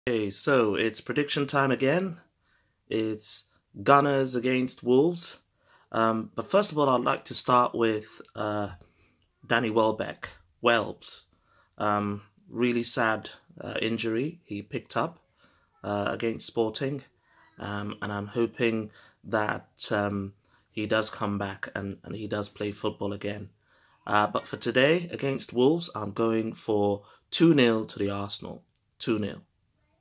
Predict Arsenal VS Wolves
EJ shouts out to Welbz - and Predicting Wolves!

Fans can Predict the score for upcoming Arsenal game with a Selfie video!!